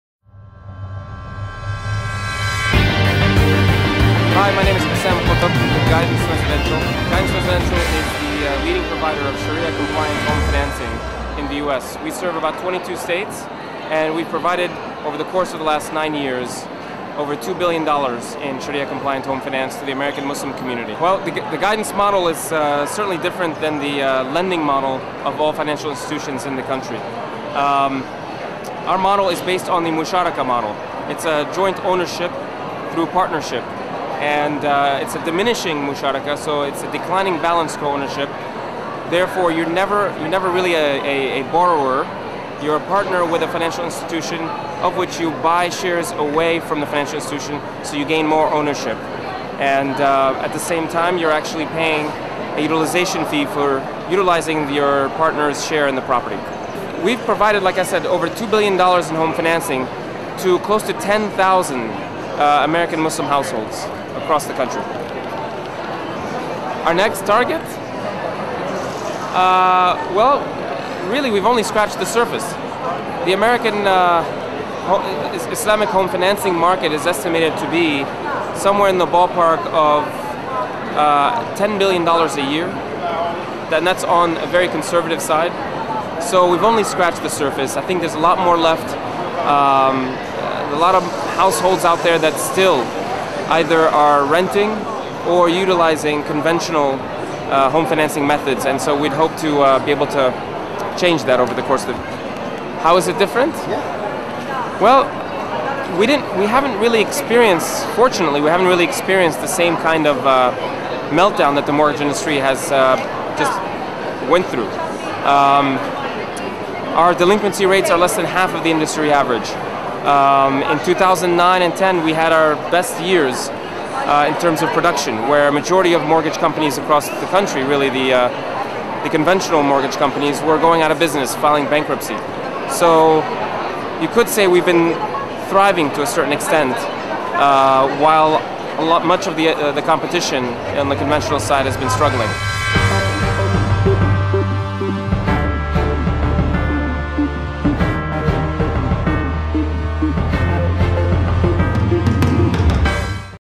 Guidance Residential, LLC.mov
Guidance Residential, LLC USA provides Islamic mortgage to Muslims in America. It has already provided up to 2 billion for 10,000 homes. Guidance Reliance, LLC is based in Reston, VA , and  do business in 22 states across USA. There is an estimated 10 billion annual mortgage market for Muslims across America. Guidance Residential,LLC. is a thriving concept in US current melt down mortgage market.
For your peace of mind and Sharia compliance mortgage details check